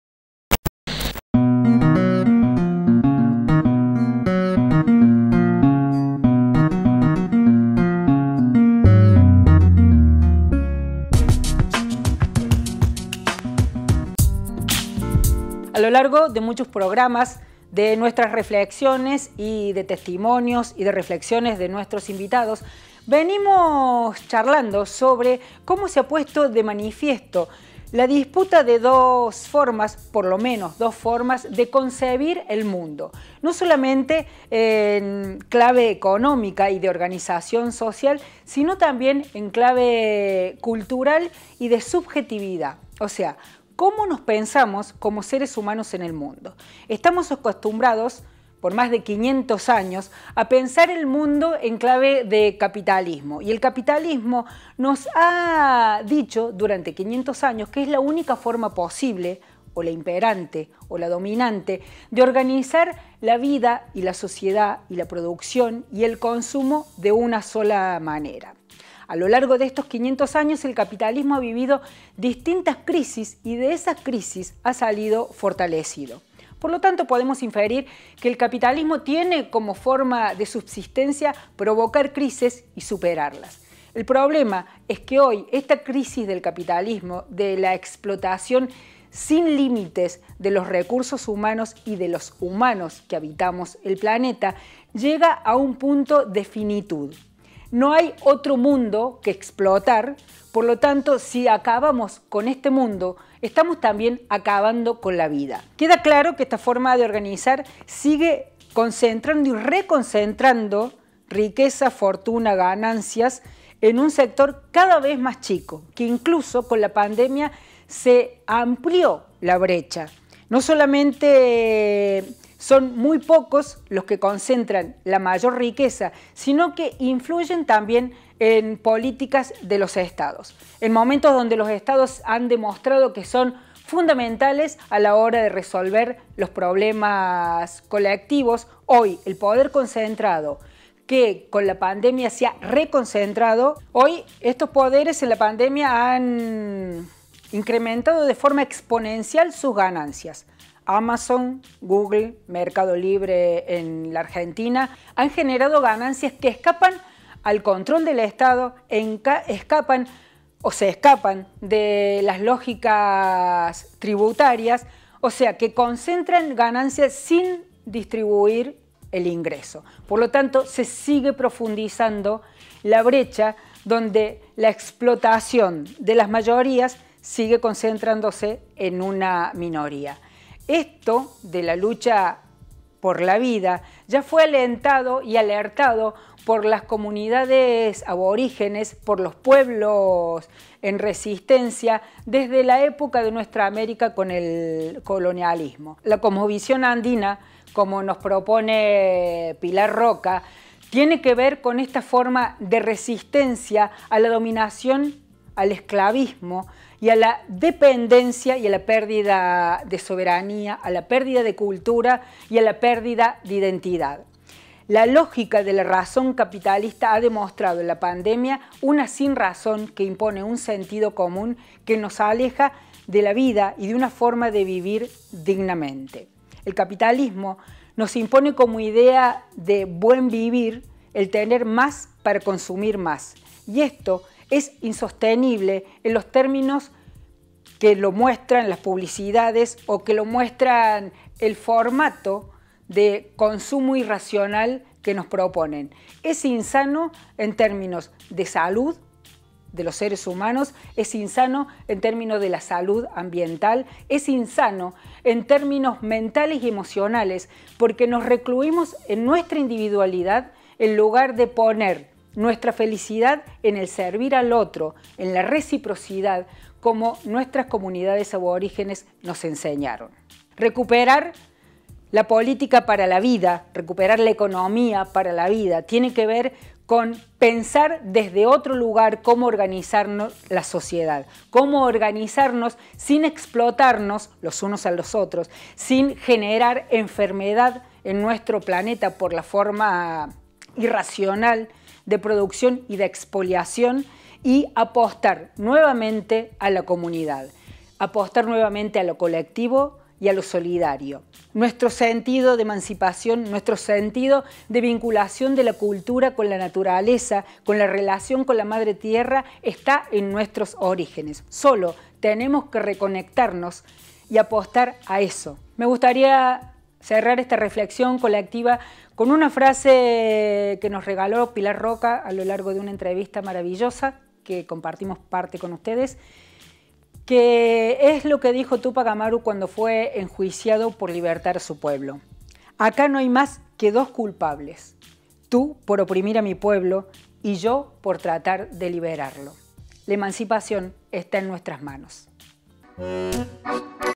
Editorial 469 Modelos en disputa
Reflexionamos sobre la concentración de riquezas y la necesidad de  crear otros modelos económicos y culturales sustentables ambiental y socialmente